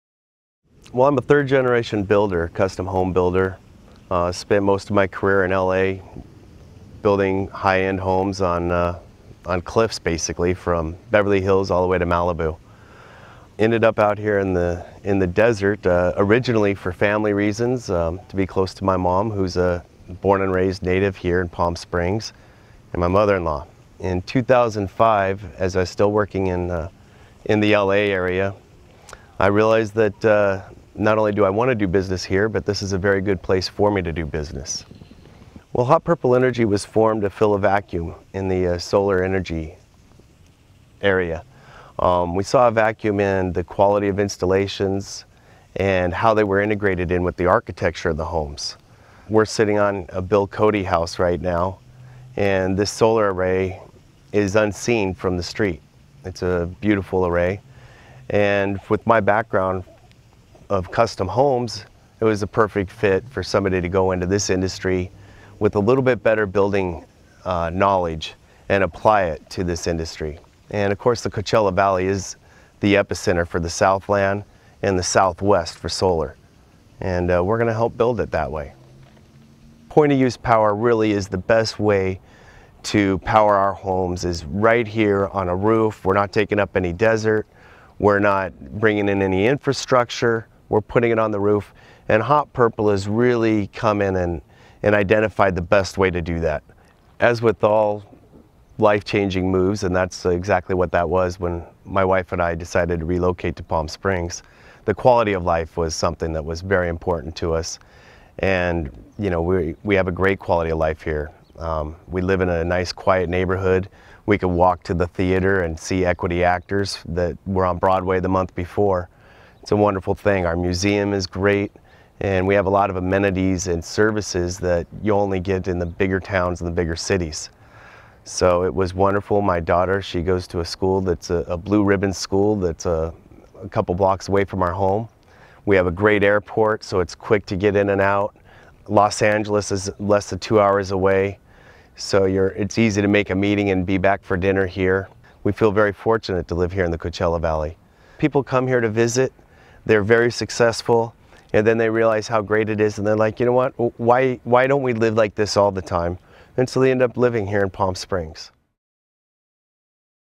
Nathan Otto - Hot Purple Energy
A CUSTOM-HOME BUILDER, Nathan Otto found inspiration in SunLine Transit Agency's clean-fuels buses. "SunLine opened my eyes that this valley was going to be an epicenter for green energy," he says. "Where we thought we could help is bringing construction knowledge into the mix for point-of-use power." Otto founded Hot Purple Energy to design residential solar systems that match the aesthetics of great architecture while meeting the current and future needs of residents.